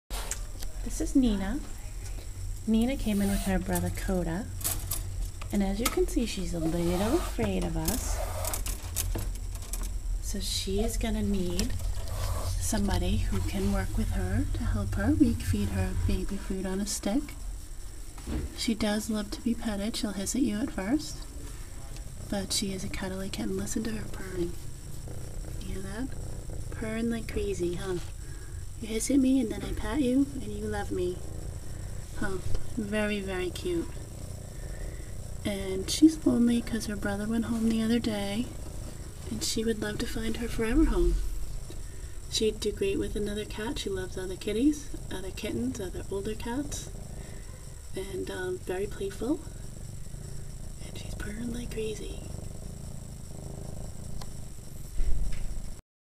Nina.MOV
Nina is an available cat at the Animal Resue League in Conway, NH